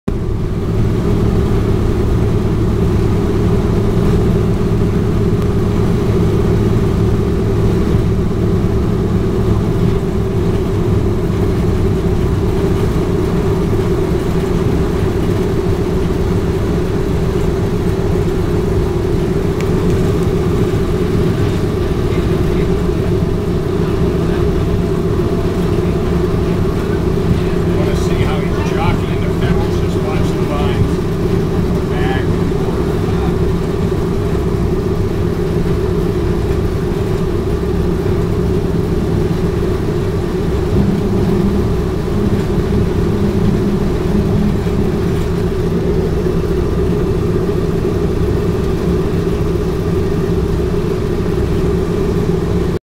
B-17 Flight: Taking off part 2
My trip on the "Nine 0 Nine" B-17 Flying Fortress